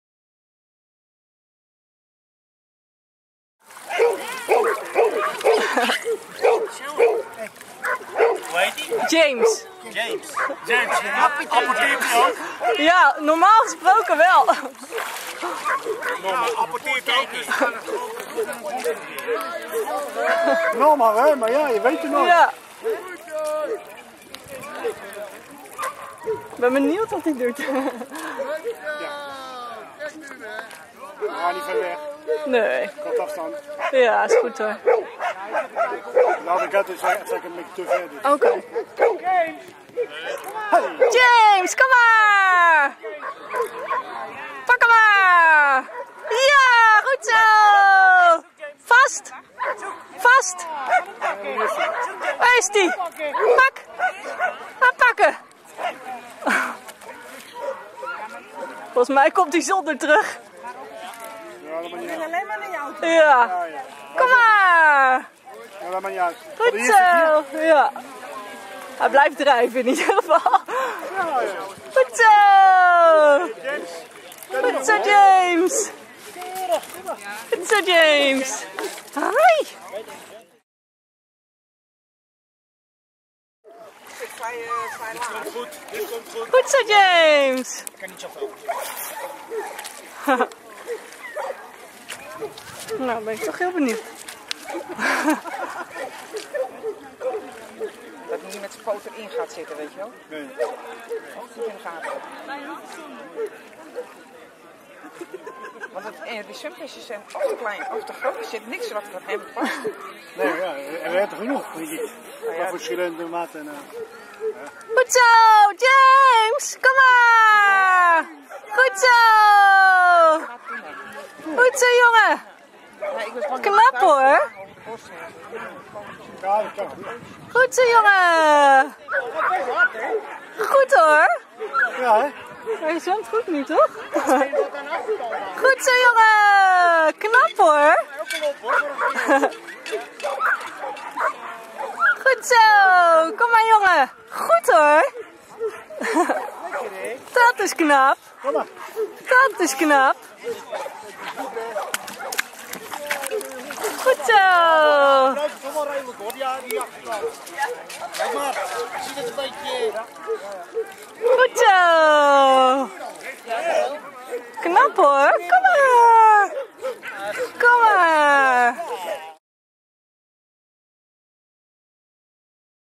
James & the first steps in water work
James (Waggerland Cape Cod) the Cardigan Welsh Corgi learning the first steps in water work! First he learns to swim a distance, later he learns how to pull a life buoy and the next step is rescuing someone who's hanging on the buoy.